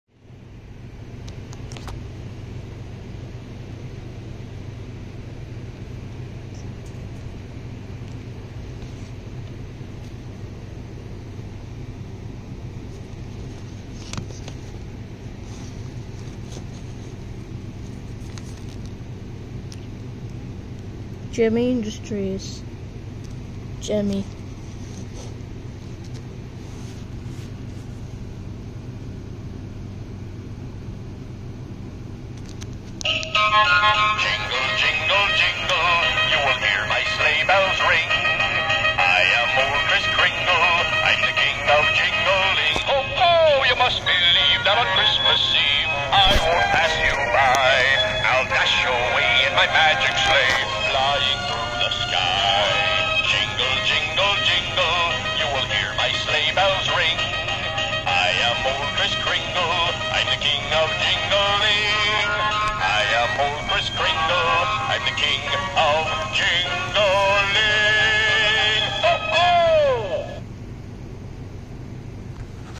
gemmy 2004 festive fellas Santa
this guy didn't need no mechanical issues fully working